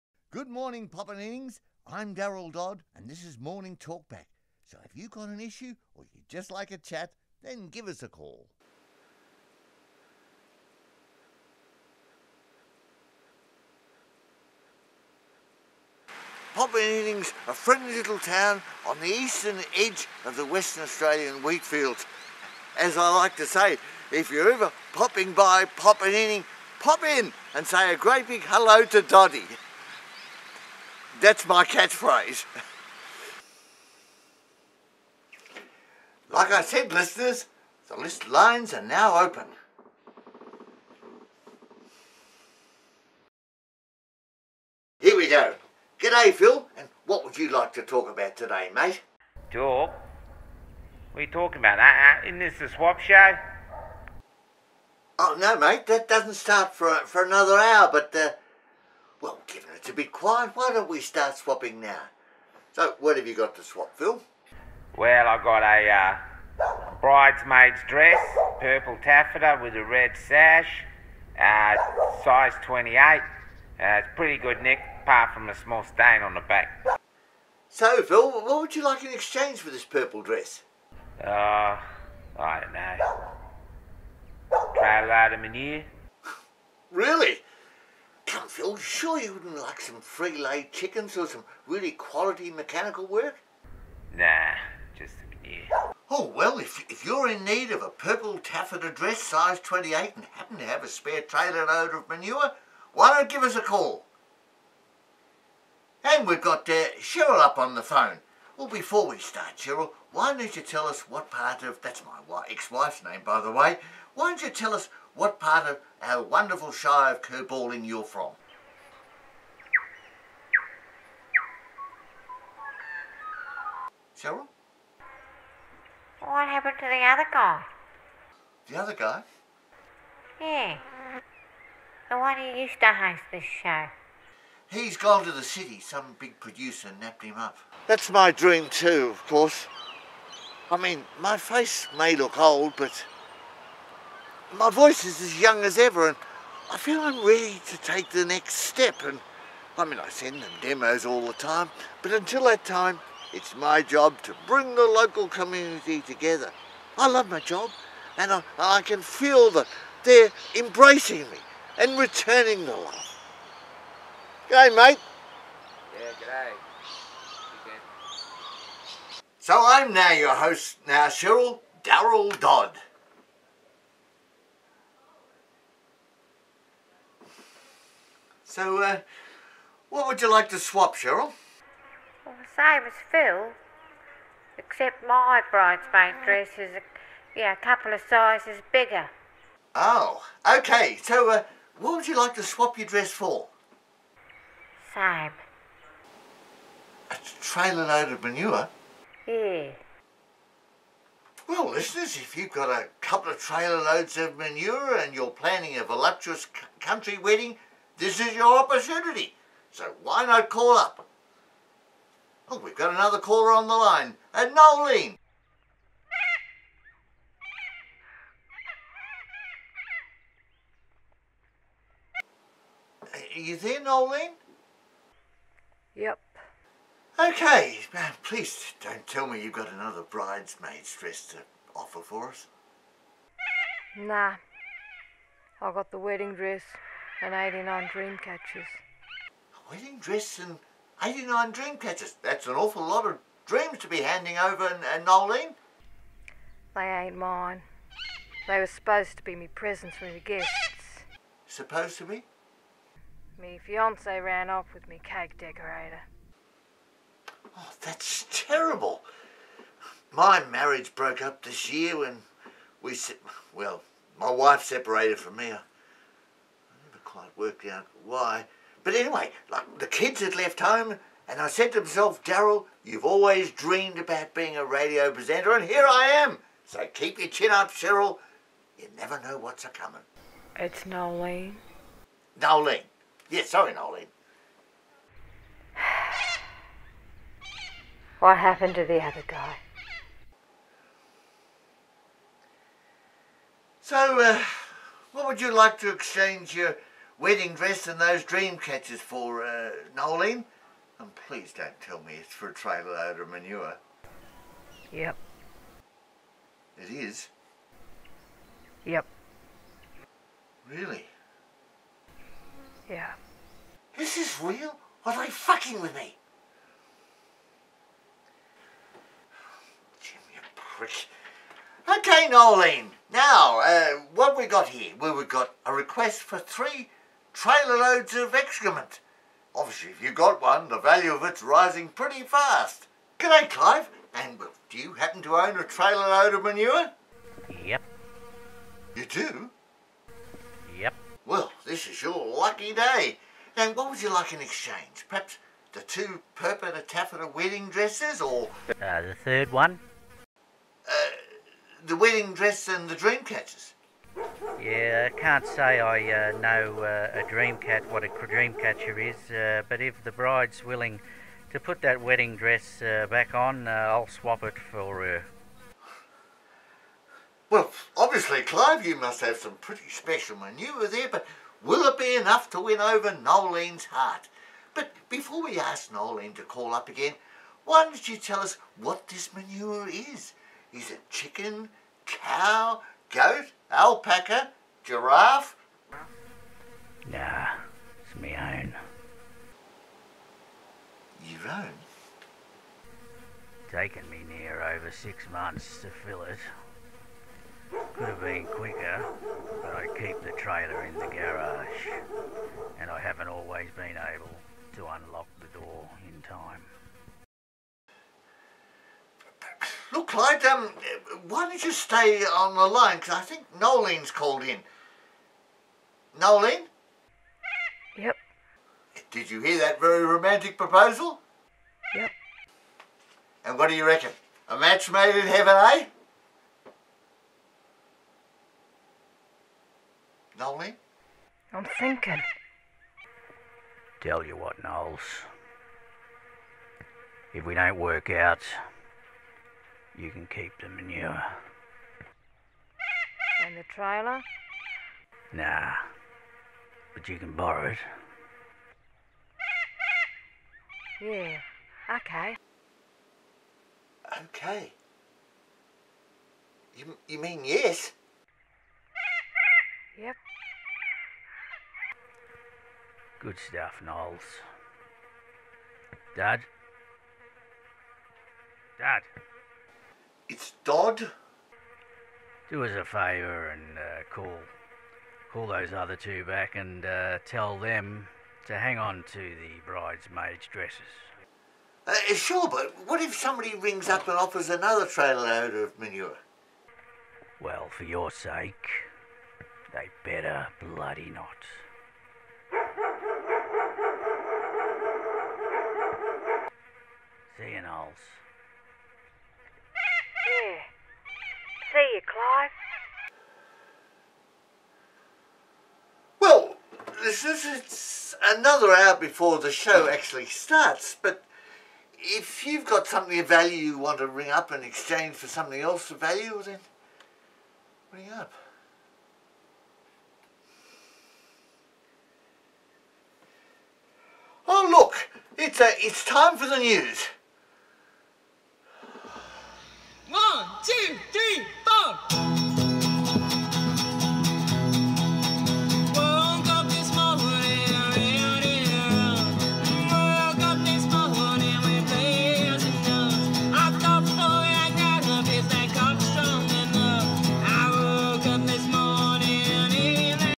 A Trailer Load of Manure
After his marriage fell apart Daryl Dodd set about achieving his life long dream to be DJ.

Now a radio presenter in the country town of Popanyinning, he is trying his best to crack the big time.

All he has to do is build a rapport with the locals.

Grizzlements is a new community for 45 plus Creatives. Our aim, have some fun and comically shake up the growing bullshit of our times . . .